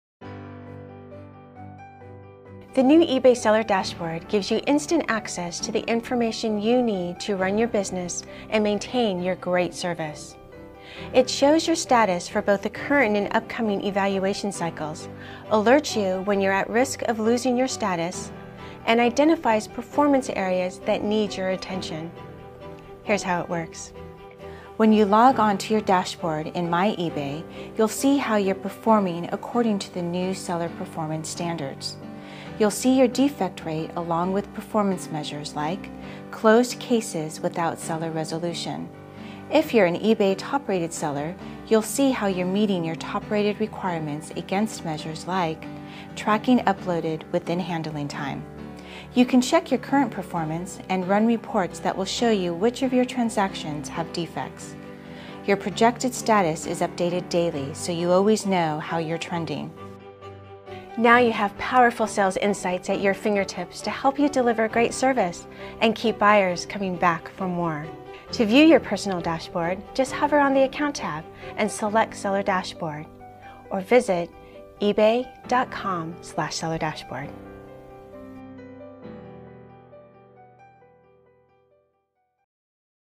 Meet the New eBay Seller Dashboard
Cheryl Fujii of eBay walks you through the new eBay Seller Dashboard. The dashboard has been completely revamped to provide all eBay sellers with the information they need to deliver great service. This easy to use tool clearly displays your status for both the current and upcoming evaluation cycles, and alerts you when you're at risk of losing your status (or not meeting eBay minimum standards), along with any performance areas that are in need of attention.